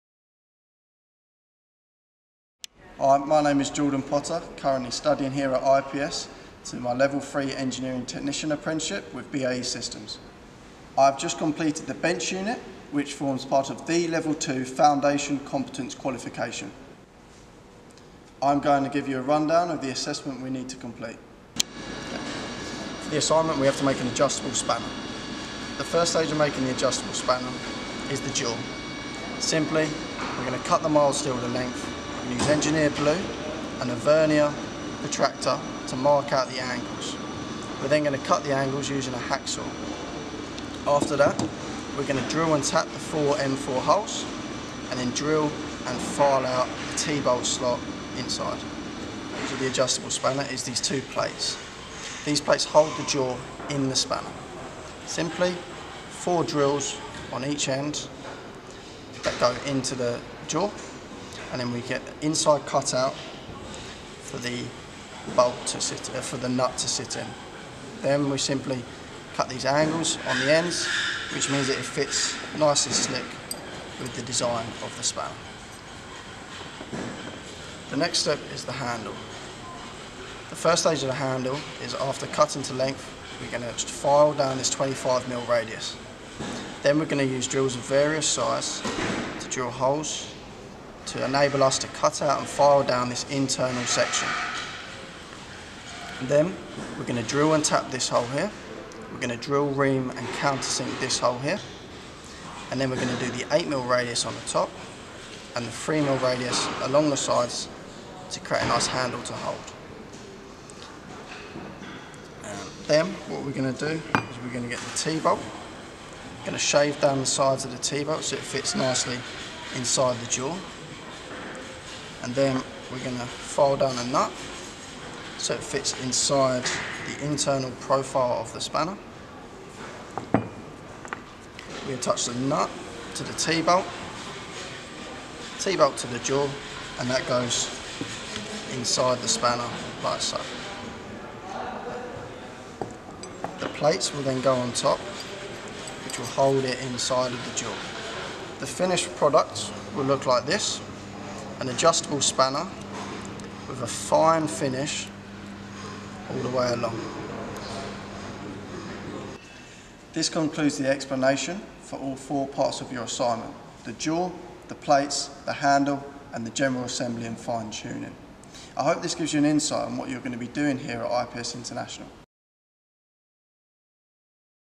BAE Systems Engineering Technican Apprentice's snippet to bench unit at IPS
Engineering Apprentice of BAE Systems, Jordan Potter, is completing the Level 3 Engineering Technician Apprenticeship. Jordan has just completed his bench unit in the IPS Workshop and explains in the video the process of this - The day in the life of the IPS Workshop and one of the kinds of thing you could learn if you complete an Engineering Apprenticeship.